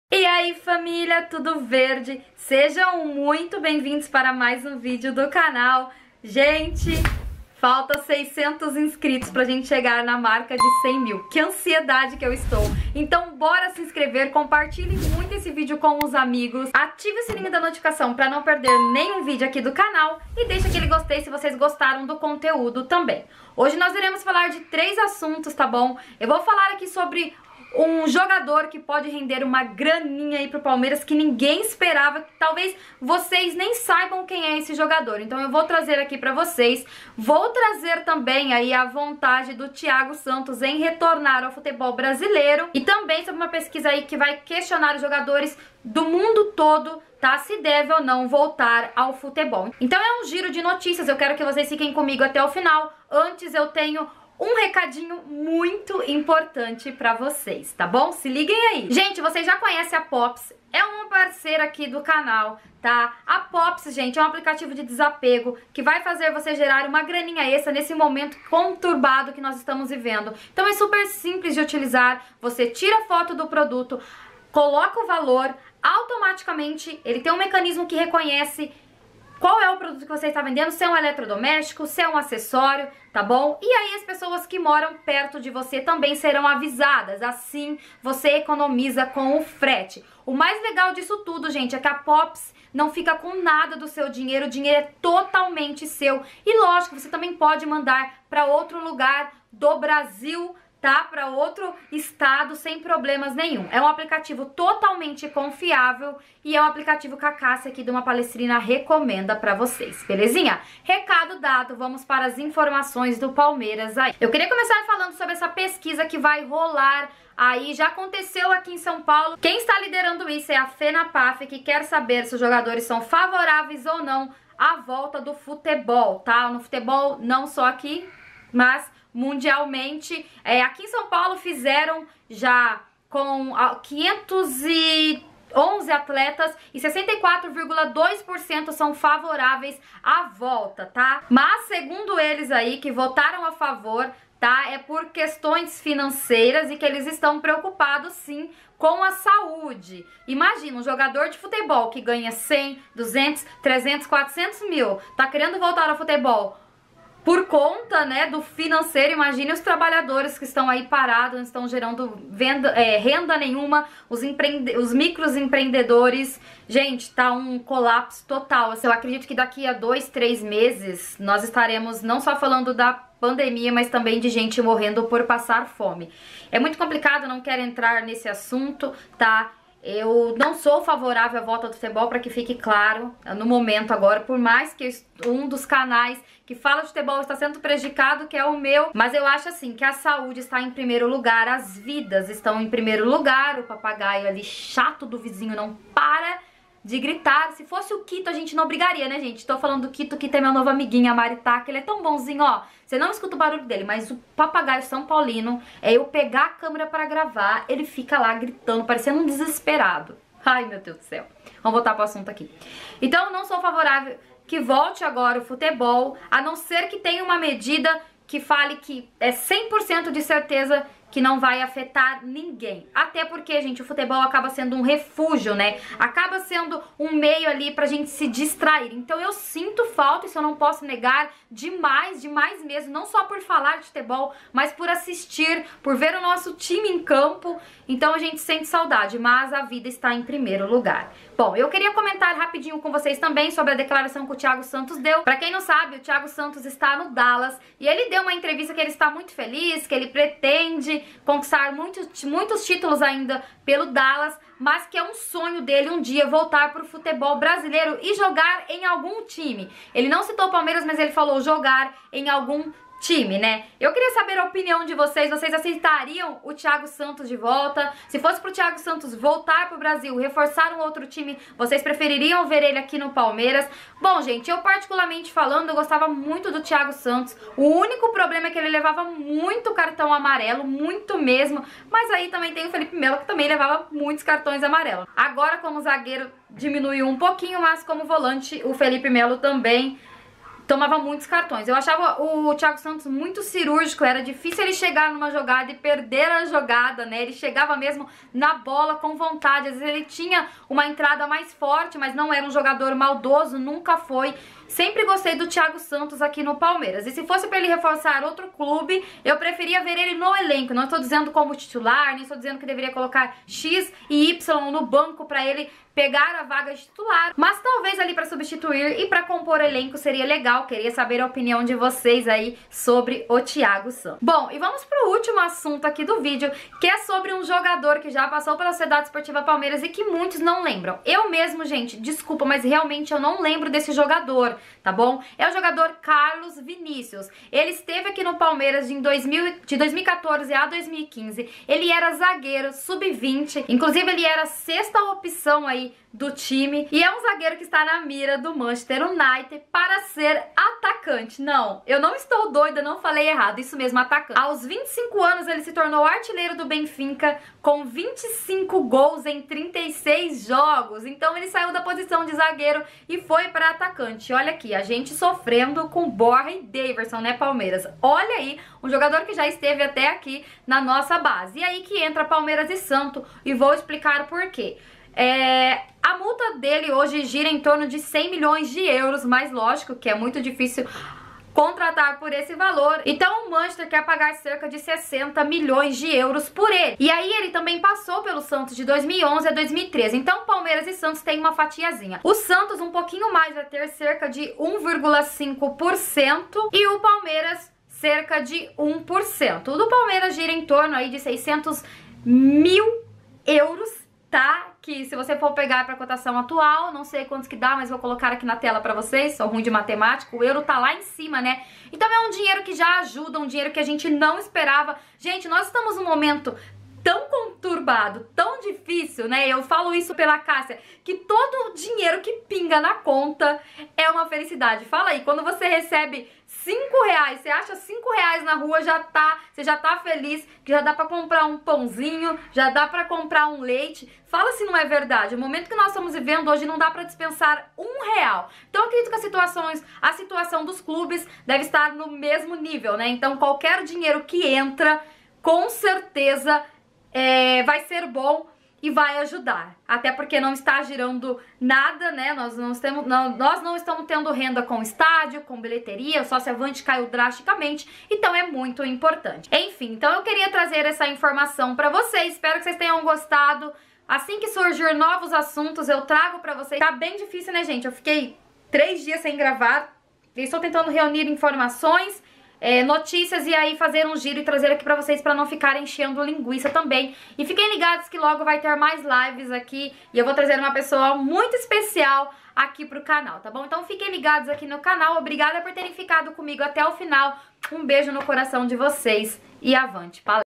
OLHA ISSO, JOGADOR "ESQUECIDO" PODE RENDER $$ AO PALMEIRAS | THIAGO TEM SONHO DE VOLTAR
Conta Itaú :
Kassia Alves Poggi
AG: 2078
CONTA: 34889-9